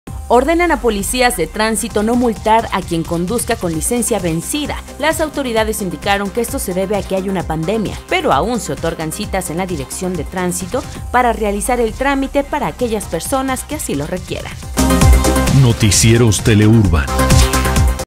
Ordenan a policías de Tránsito no multar a quien conduzca con licencia vencida
Las autoridades indicaron que esto se debe a que hay una pandemia, pero aún se otorgan citas en la Dirección de Tránsito para realizar el trámite "para aquellas personas que así lo requieran".